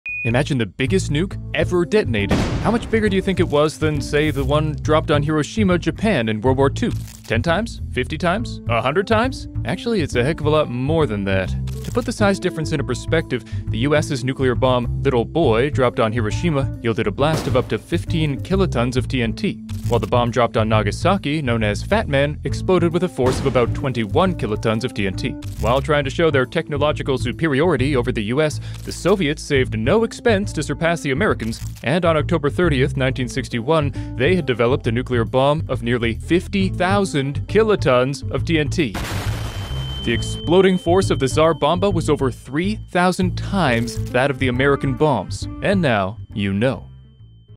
The Largest Nuke Ever Detonated
What is the largest nuke ever detonated? Well, a nuclear bomb that the Soviets created, and detonated surprisingly enough named the Tsar Bomba. This Nuke was so much larger than the WW2 Hiroshima and Nagasaki bombs that it's hard to even compare them - although we tried! It's much larger than the mother of all bombs, too.

Original script by Thales Cardozo